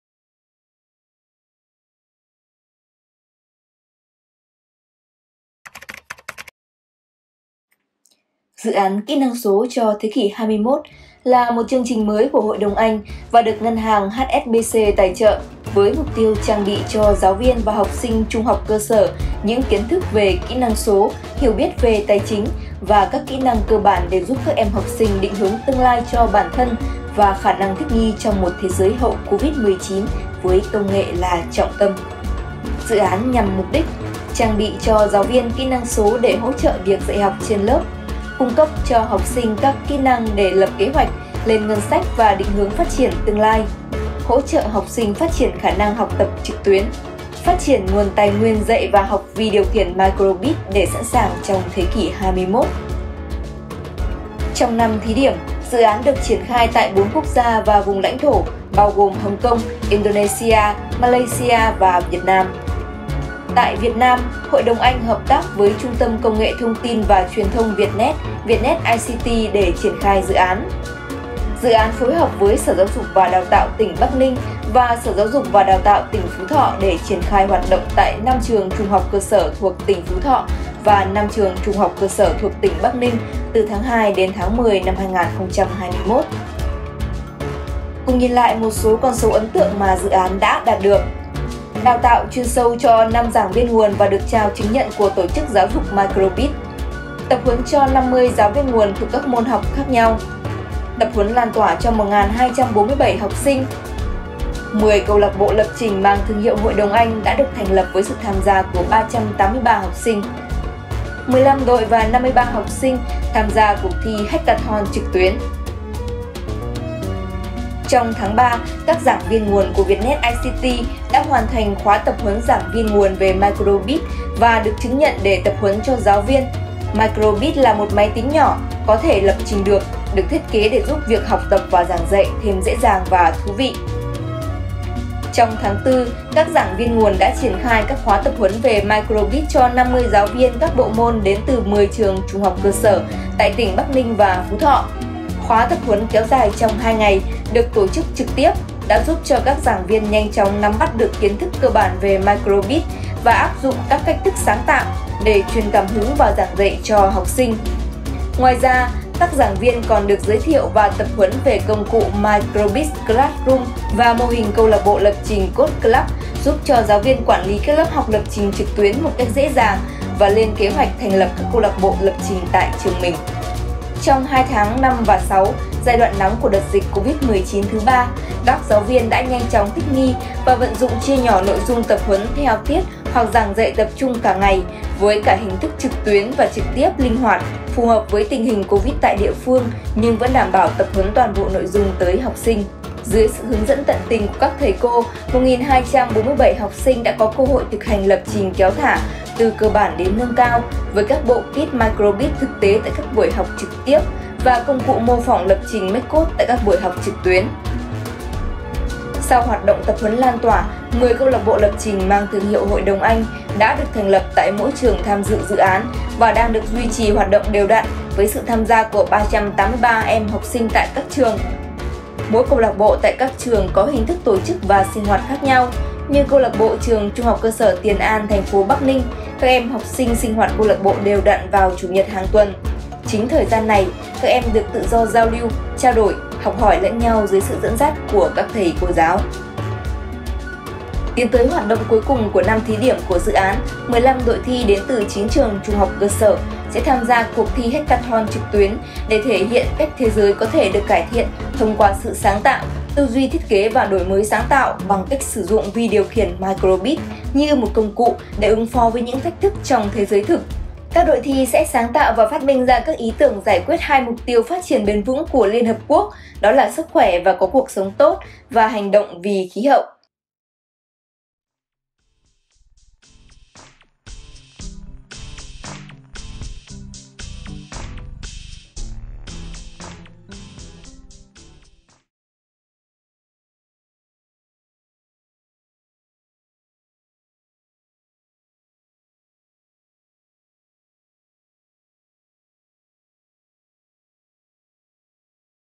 TỔNG KỂT DỰ ÁN KỸ NĂNG SỐ CHO THẾ KỶ 21 - The Digital Readiness Academy
Dự án Kỹ năng số cho thế kỷ 21 (The Digital Readiness Academy) là một chương trình mới của Hội đồng Anh và được Ngân hàng HSBC tài trợ với mục tiêu trang bị cho giáo viên và học sinh trung học cơ sở những kiến thức về kỹ năng số, hiểu biết về tài chính và các kỹ năng cơ bản để giúp các em học sinh định hướng tương lai cho bản thân và khả năng thích nghi trong một thế giới hậu Covid-19 với công nghệ là trọng tâm.

The Digital Readiness Academy is a new programme of the British Council funded by HSBC with the aim of empowering local junior secondary-school teachers and students with digital literacy, financial literacy and core (soft) skills for their future planning and resilience in a post-Covid-19 and tech-focused world.